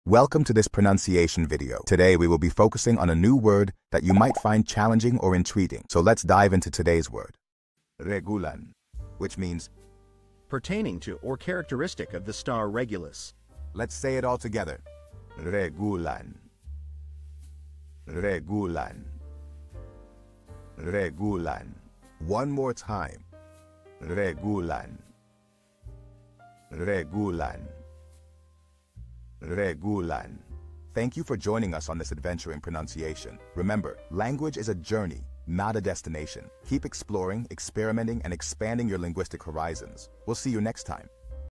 How to pronounce Regulan
Master the Pronunciation of 'Regulan Which Means regulate' - which means : Pertaining to Orcteristic of the Star Regulus. 📝 with @PronunciationSpanish 🎓🗣️ - [Your Guide to Spanish]

Learn to pronounce 'Regulan' perfectly in Spanish with our easy and engaging tutorial by @PronunciationSpanish! 📖📽️ In this comprehensive video, we provide you with native speaker examples 🗣️👂 and phonetic breakdowns 🎵 to ensure you get the pronunciation just right.

How to say Regulate in Spanish